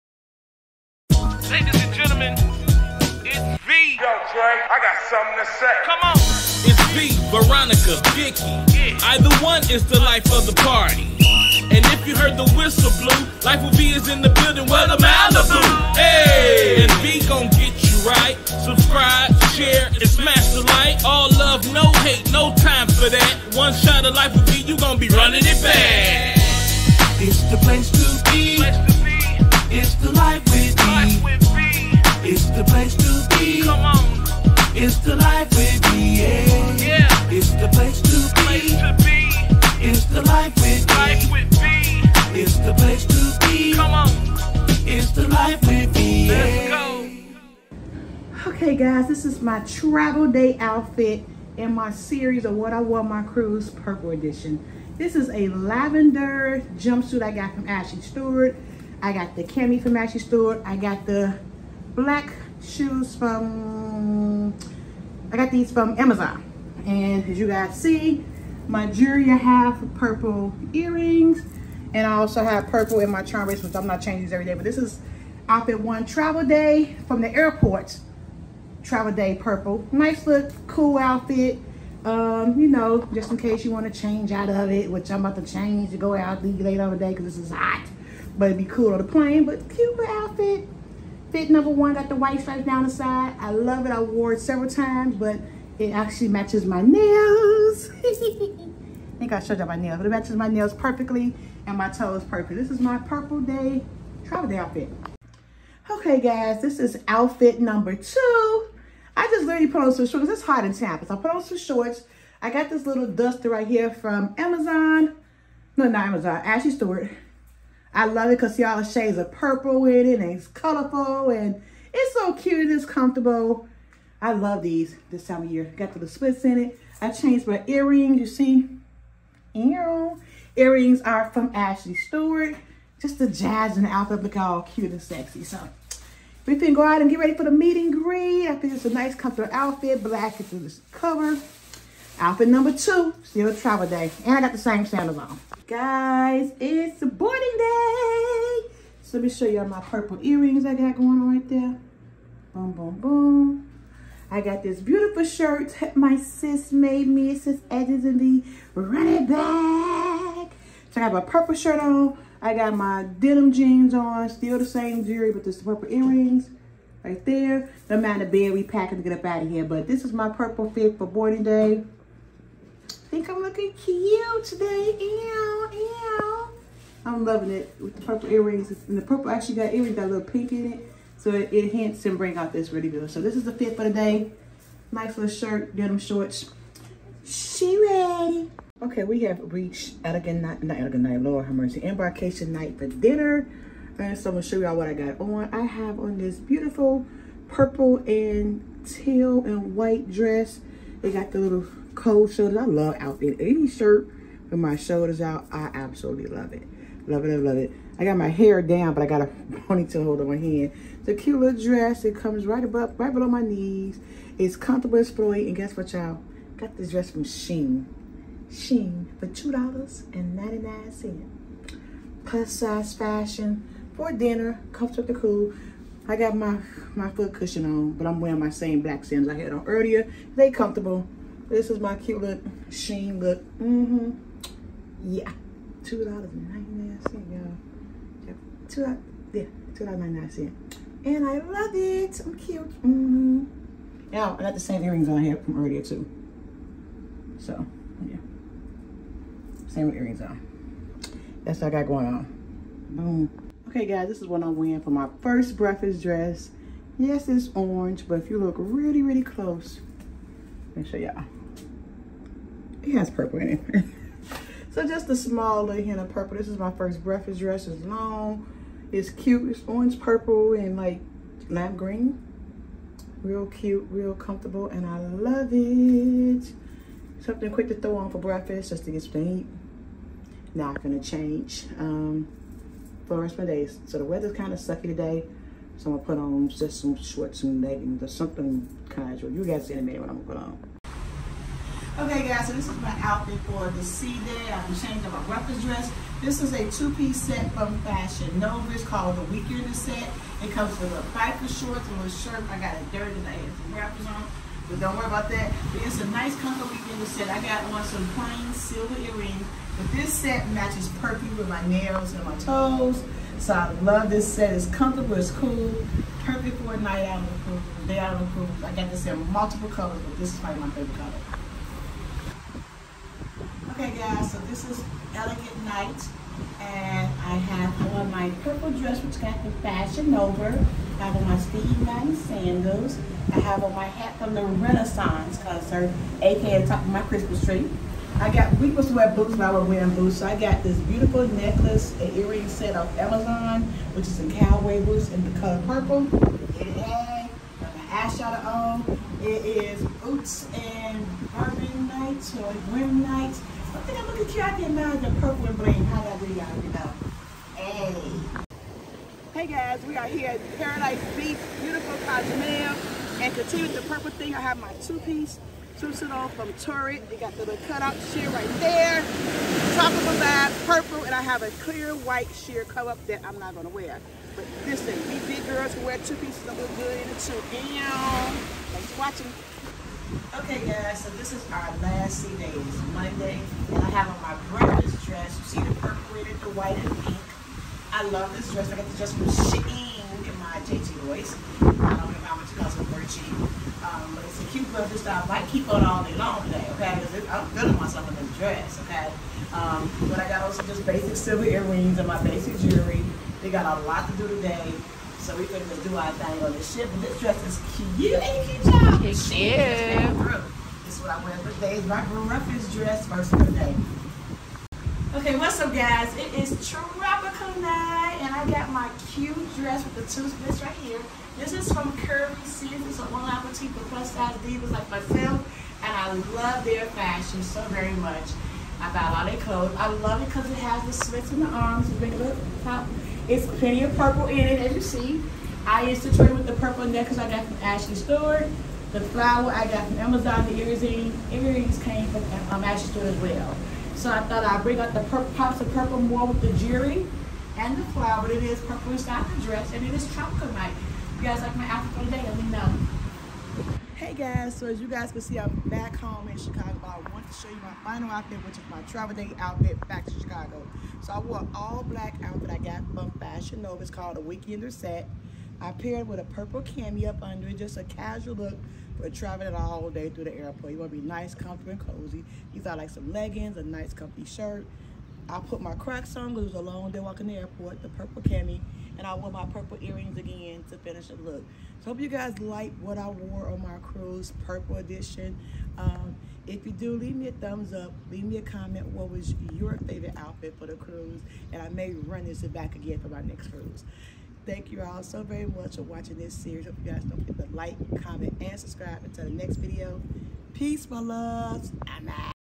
WHAT I WORE ON MY CRUISE PLUS SIZE EDITION 2024: Fashion Nova, SHEIN, Amazon, Curvy Sense & Torrid
Plus size fashion I wore on my cruise with a twist. Shades of Purple.

WANT TO SEND ME SOMETHING:
Life with V
Glenwood, Illinois 60425